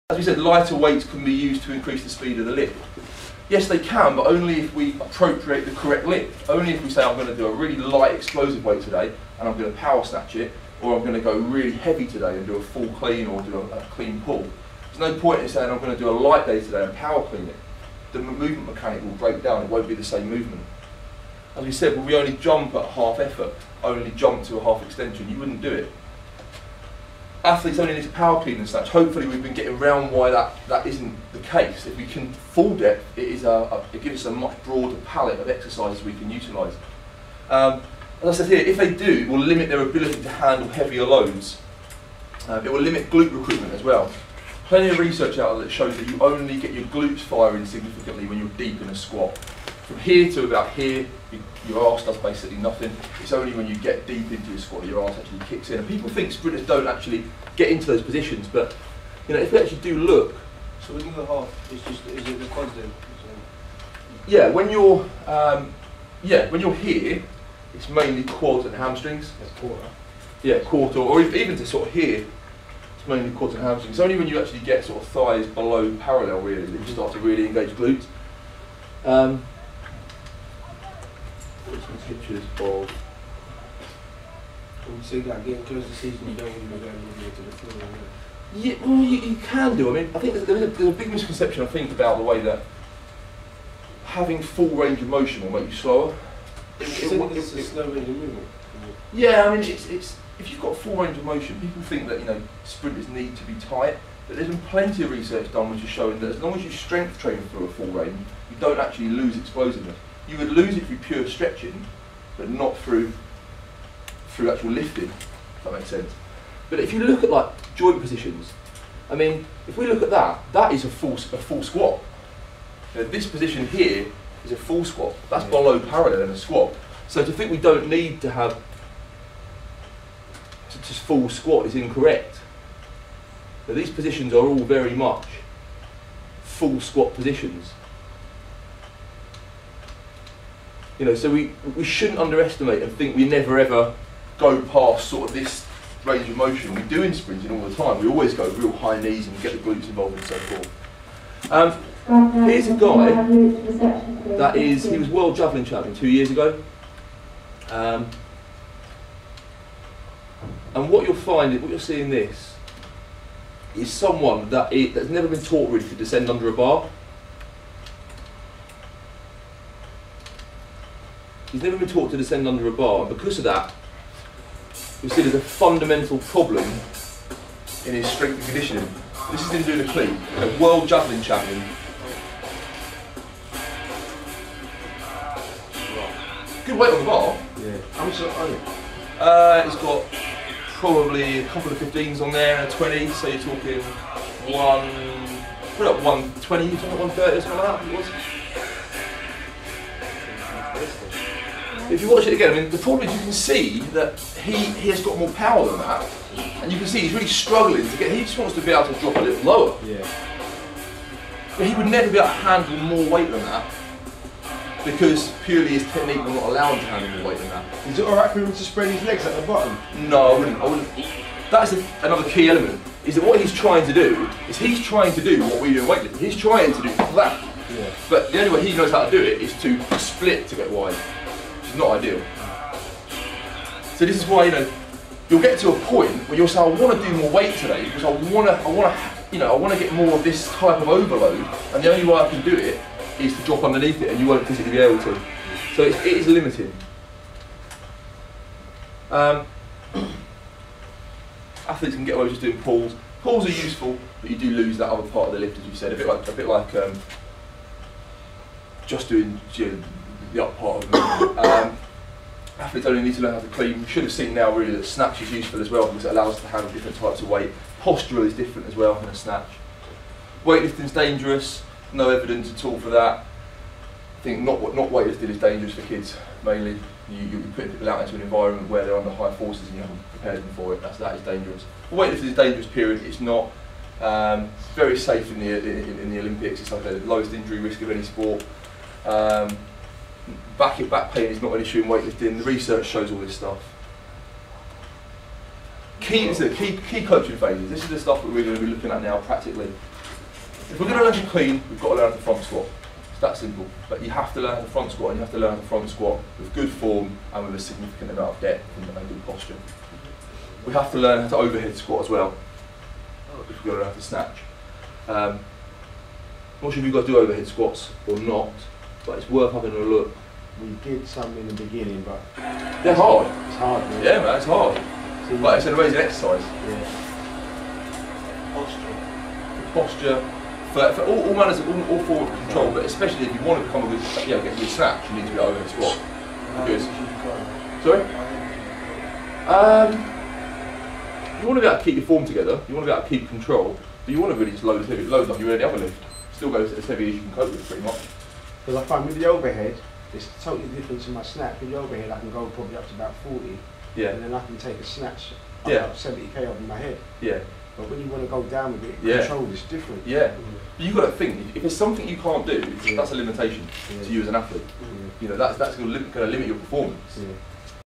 Olympic Weightlifting for Sprinting Part 5
Phil Lectures Top Coach Sprint Squad on why and how Olympic Weightlifting is beneficial to Sprinters.